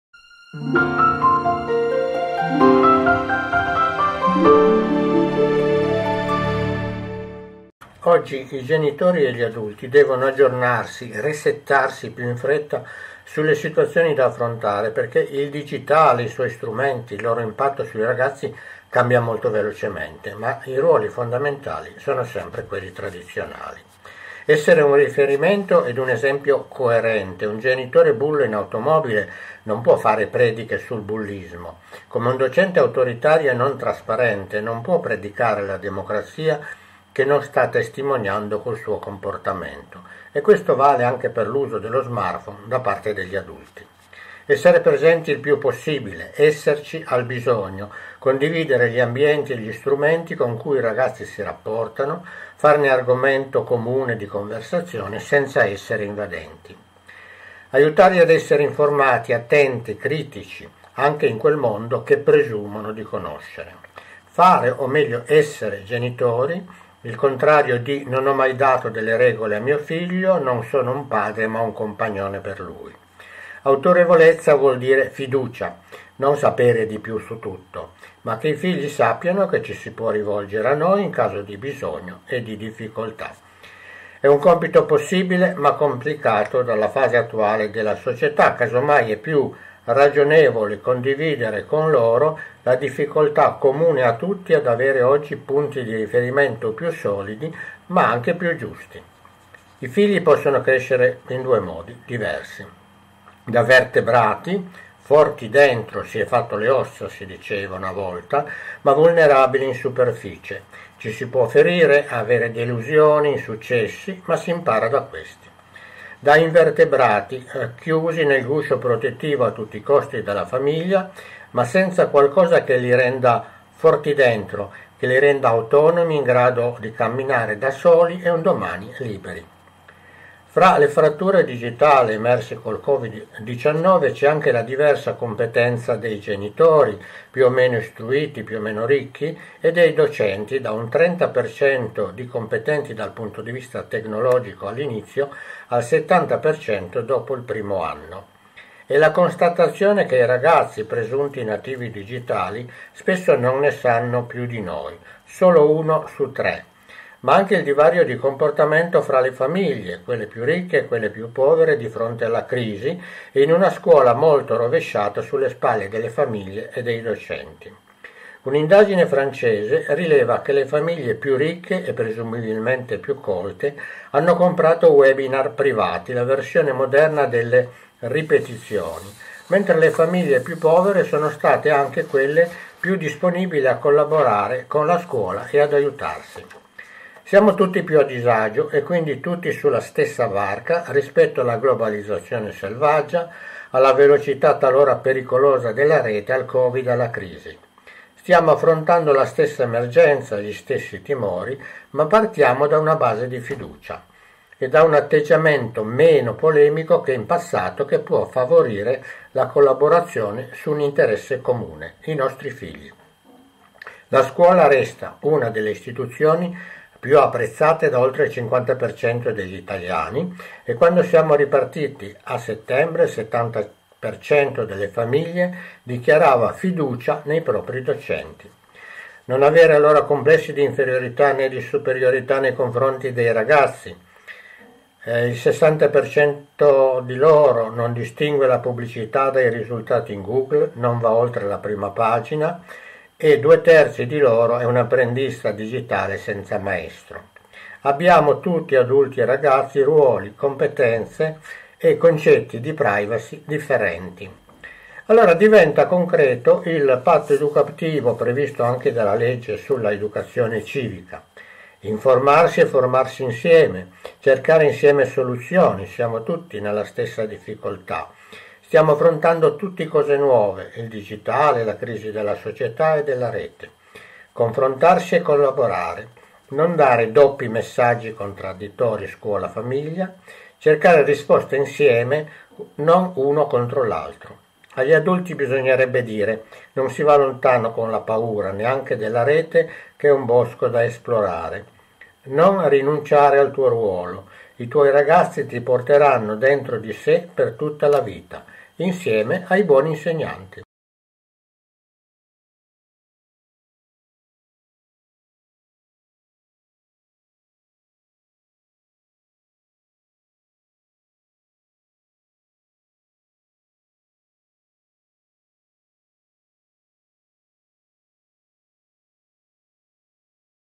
Educazione digitale: collaborazione scuola e famiglia
tecnicadellascuola sosgenitori cittadinanzadigitale Tra le fratture digitali (attrezzature, banda, competenze digitali e possibilità della famiglia) emerse col Covid 19, c’è anche la diversa competenza dei genitori (più o meno istruiti e colti, più o meno ricchi) e dei docenti (da un 30% di competenti dal punto di vista tecnologico all’inizio, al 70% dopo il primo anno di DaD).

A cura di Rodolfo Marchisio